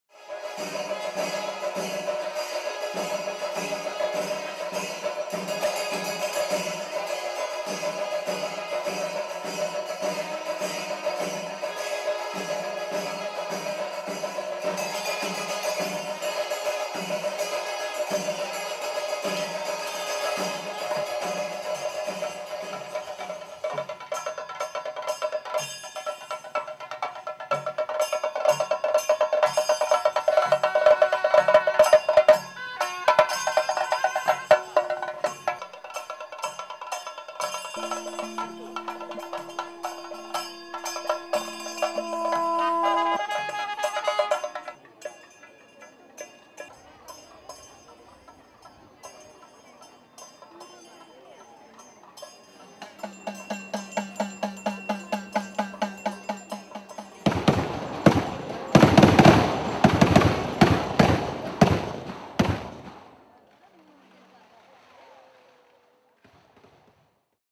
Vela at Mangottu Bhagavathi Temple in Palakkad, Kerala
Mangottu Bhagavathi Temple is a famous shrine with sovereignty of Olappamanna Mana.

To see the Festival Calendar with details of 50 monsoon festivals please